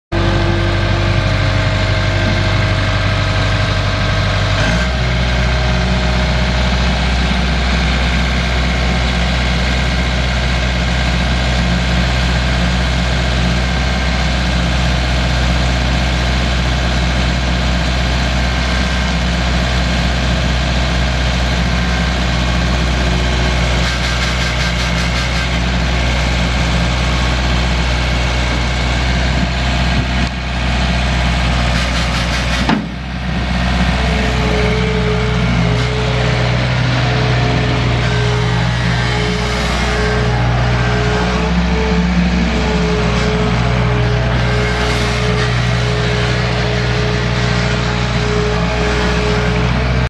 1996 Case 1845C skid steer for sale at auction | bidding closes October 15, 2019
1996 Case 1845C skid steer
3,433 hours on meter
Cummins 4BT four cylinder diesel engine
Hydrostatic transmission
OROPS
Hand bucket controls
Auxiliary hydraulics
Manual quick coupler
72"W bucket
Counter weights
Galaxy 12x16.5 tires
Additional tire and wheel
(2) parts/owners manuals
Maintenance records available upon request
Serial JAF0192412
Unit# 2406